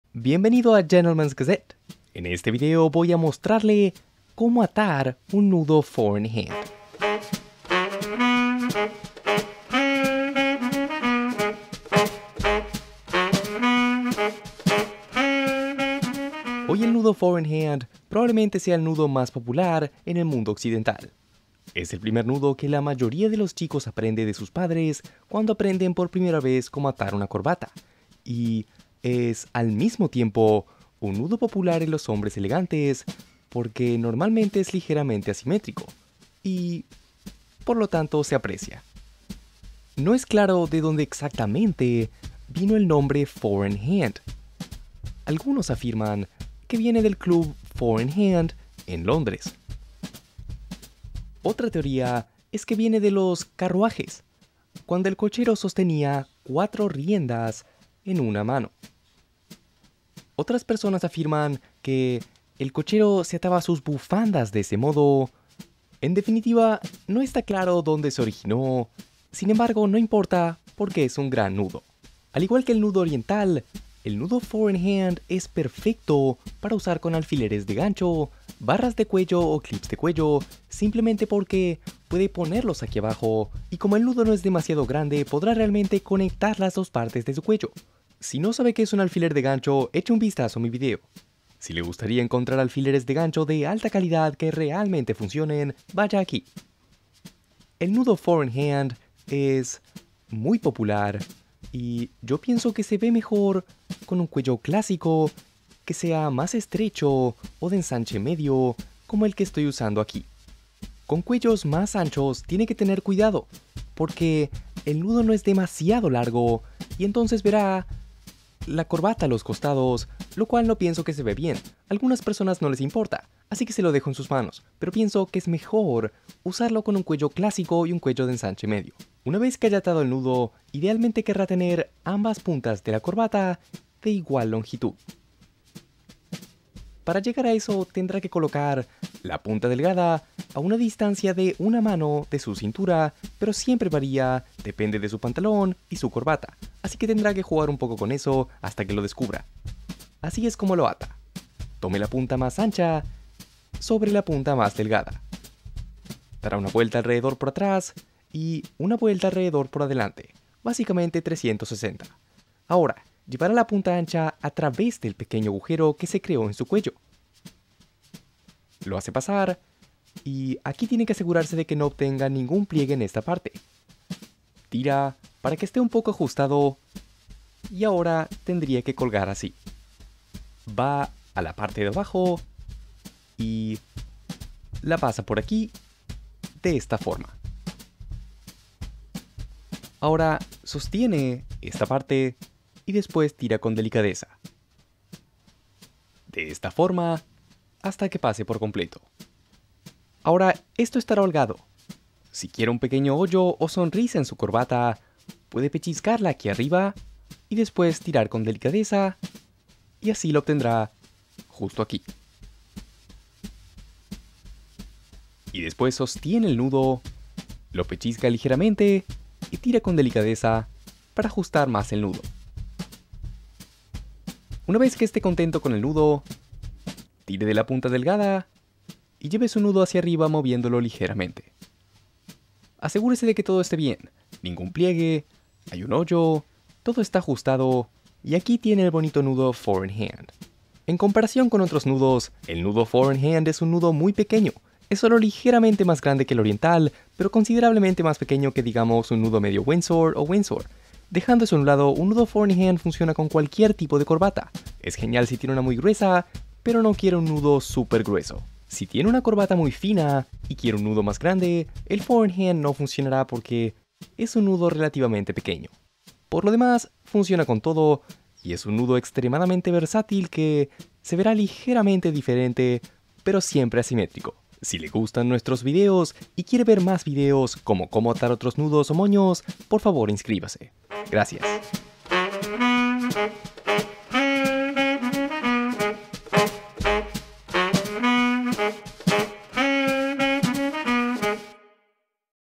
Tutorial del nudo de corbata four in hand - Guía paso a paso
Aprenda cómo atar el nudo four in hand de la manera fácil con instrucciones paso a paso para que su nudo se vea genial y no descuidado

→ Corbata estampada de seda madder amarilla con patrón de diamantes rojo, azul y naranja
→ Corbata de seda borilla con patrón pata de gallo en verde botella y gris perla

GUÍAS QUE NO QUERRÁ PERDERSE:
→ Los 3 nudos de corbata más fáciles para principiantes - Instrucciones paso a paso:

→ Traducción y doblaje: Martín Petrera

El four in hand es un nudo popular en los hombres elegantes porque normalmente es ligeramente asimétrico y no tan grande, por lo que funciona con todas las corbatas (incluidas las de punto).

00:00 Introducción

En este video, aprenderá cómo atar el nudo four in hand con cualquier corbata.

Hoy el nudo four in hand» probablemente sea el nudo más popular en el mundo occidental. Es el primer nudo que la mayoría de los chicos aprende de sus padres cuando aprenden por primera vez cómo atar una corbata y es al mismo tiempo un nudo popular en los hombres elegantes porque normalmente es ligeramente asimétrico. Por lo tanto, se aprecia.

00:39 ¿De dónde viene el nombre?

No está claro de dónde viene exactamente el nombre «four in hand»; si quisiera saber más detalles, mire el video hasta el final.

01:37 ¿Con qué cuello se ve mejor?

El nudo four in hand es muy popular y pensamos que se ve mejor con un cuello clásico estrecho o de ensanche medio.

Con cuellos más anchos tiene que tener cuidado porque el nudo no es demasiado largo y entonces verá la corbata a los costados; lo cual Raphael no piensa que se ve bien. A algunas personas no les importa, así que se lo dejamos en sus manos.

2:08 Paso previo antes de atarlo

Una vez que haya atado el nudo, idealmente querrá tener ambas puntas de la corbata de igual longitud. Para llegar a eso, tendrá que colocar la punta delgada a una distancia de una mano de su cintura pero siempre varía; depende de su pantalón y su corbata. Así que tendrá que jugar con eso un poco hasta que lo descubra.

02:30 Cómo atarlo

04:00 ¿Para qué sirve y no sirve el four in hand?

En comparación con otros nudos, el four in hand es un nudo muy pequeño. Es solo ligeramente más grande que el oriental, pero considerablemente más pequeño que, digamos, un nudo medio windsor o windsor.

Dejando eso a un lado, un nudo four in hand funciona con cualquier tipo de corbata. Es genial si tiene una muy gruesa pero no quiere un nudo supergrueso.

Si tiene una corbata muy fina y quiere un nudo más grande, el four in hand no funcionará porque es un nudo relativamente pequeño.

Por lo demás, funciona con todo y es un nudo extremadamente versátil que se verá ligeramente diferente pero siempre asimétrico.